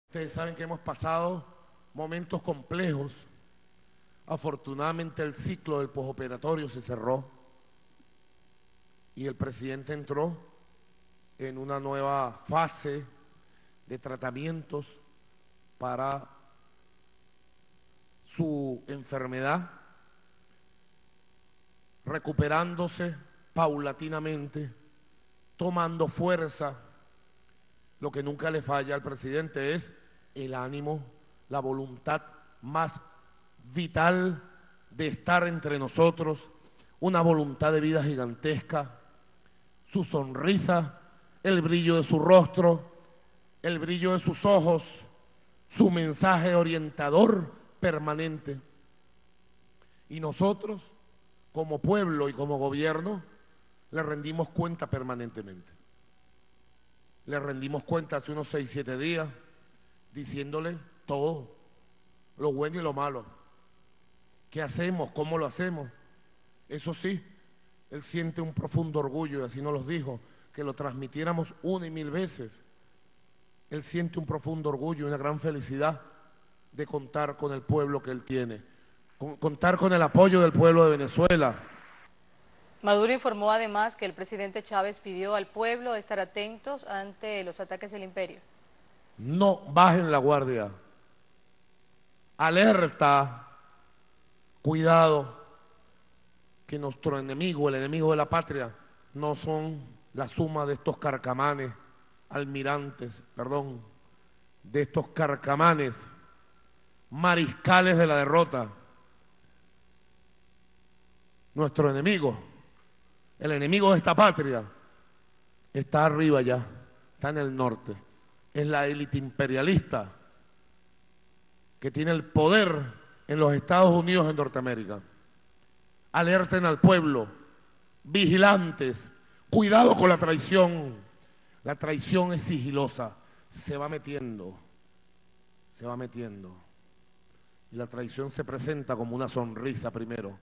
Venezuela: Nicolás Maduro informa sobre estado de salud del Presidente Chávez
El vicepresidente Ejecutivo venezolano, Nicolás Maduro, reiteró que el presidente Chávez "entró en una nueva fase de tratamiento", luego de cerrar el ciclo del postoperatorio. Trasmite mensaje al pueblo venezolano.